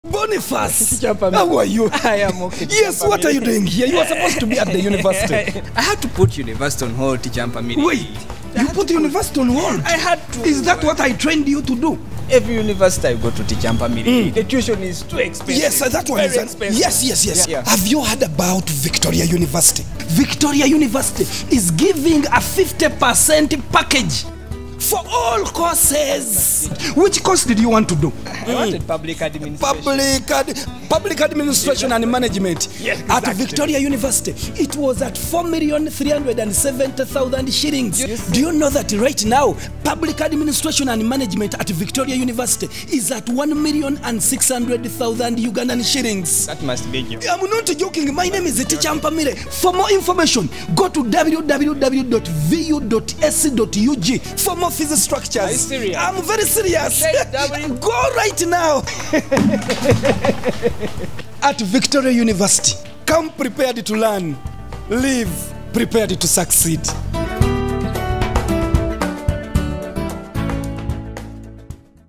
Teacher Mpamire vu half-price full value
Victoria University has made education affordable with a 50% tuition reduction Teacher Mpamire